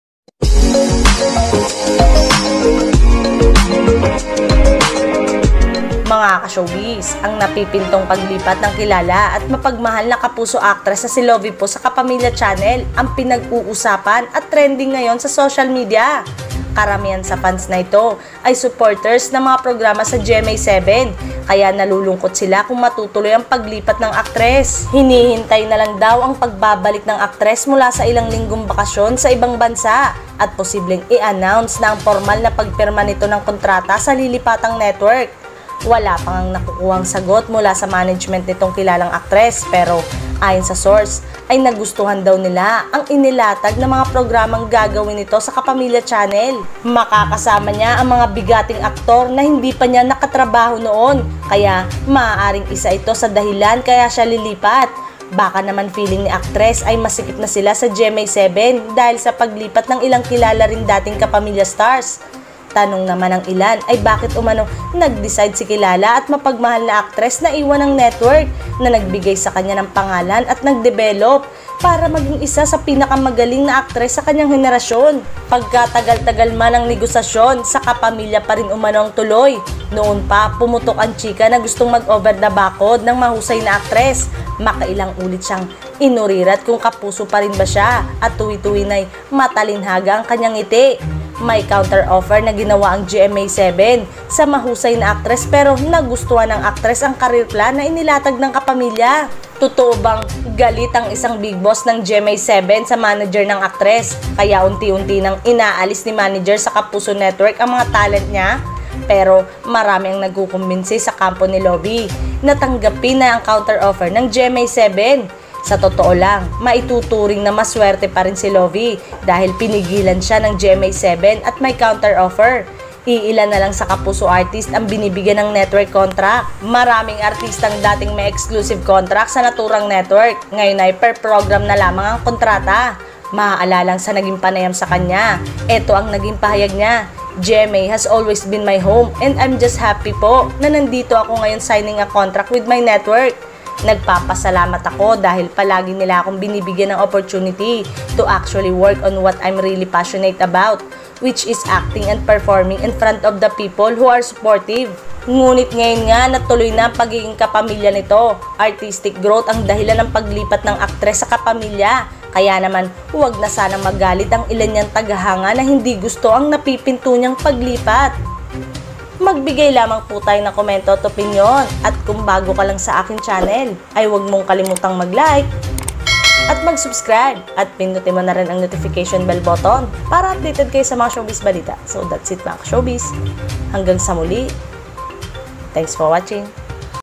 LOVI POE TULOY NA SA PAGPIRMA NG KONTRATA SA KAPAMILYA NETWORK
Ang napipintong paglipat ng kilala at mapagmahal na Kapuso actress na si lovi poe sa Kapamilya Channel ang pinag-uusapan at trending ngayon sa social media.